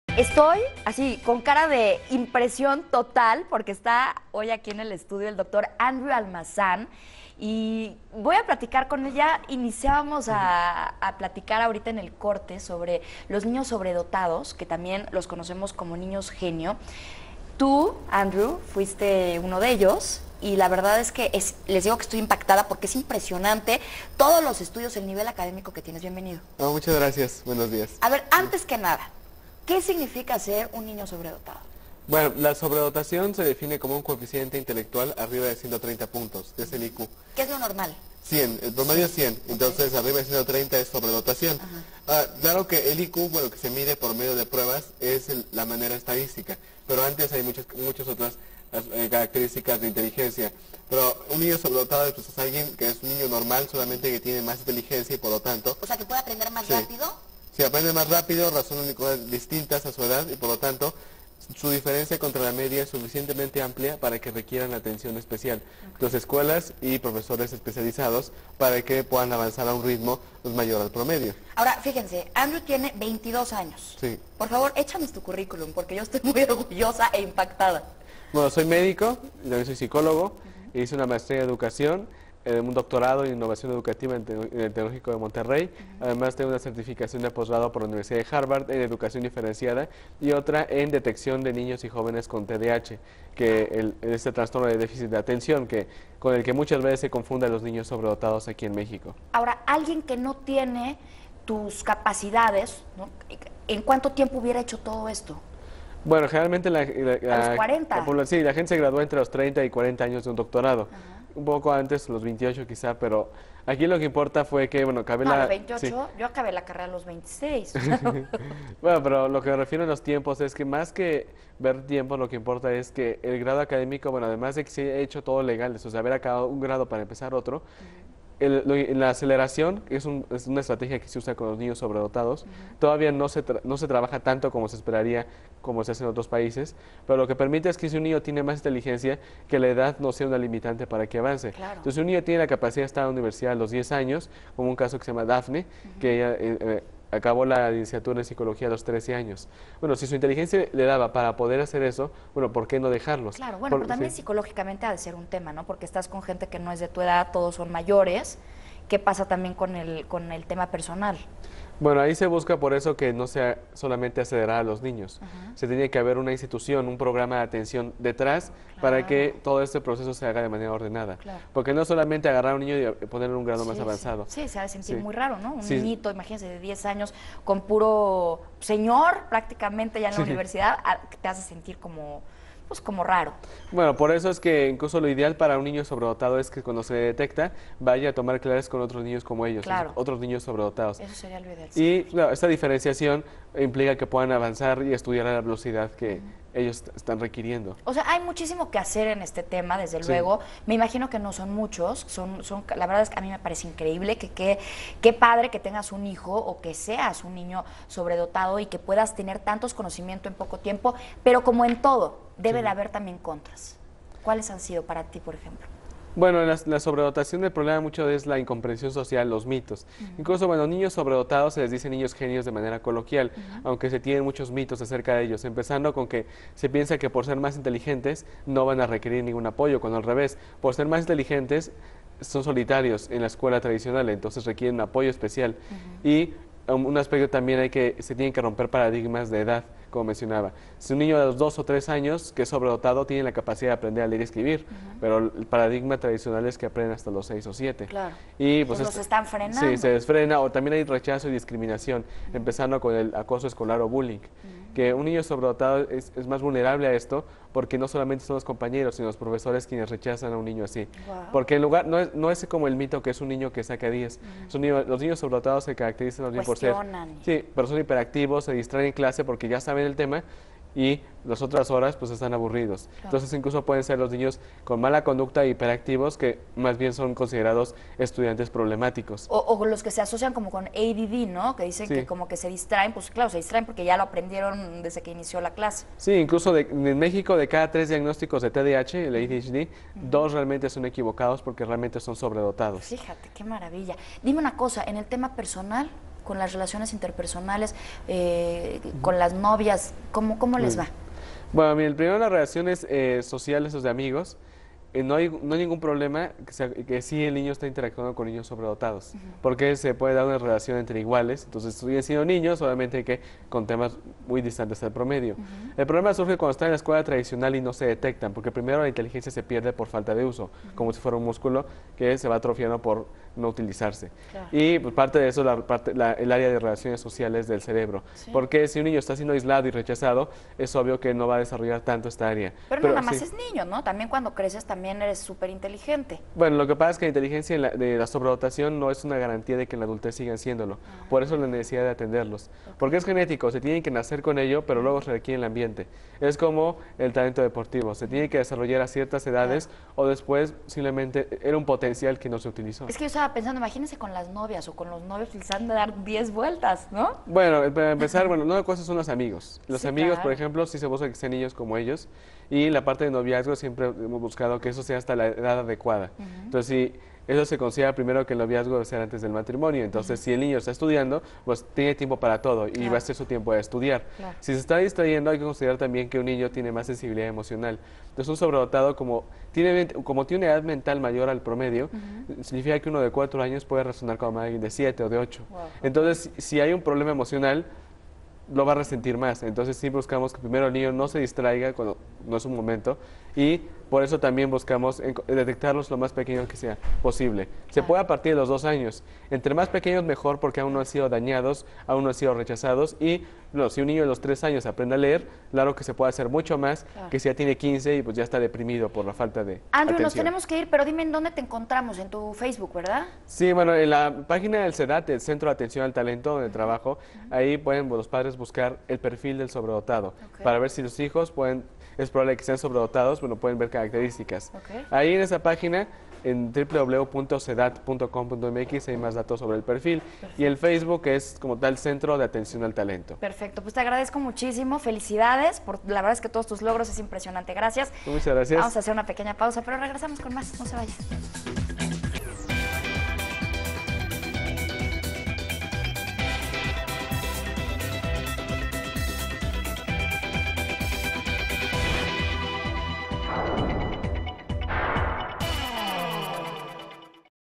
Andrew Almazán y los niños sobredotados
El joven mexicano Andrew Almazán desarrolló su inteligencia desde muy pequeño. A los 12 años de edad entró a la licenciatura de medicina y a la de psicología.  17 marzo de 2017

Conductor: Yazmín Jalil.
Horario: Lunes a Viernes @13:00-13:30.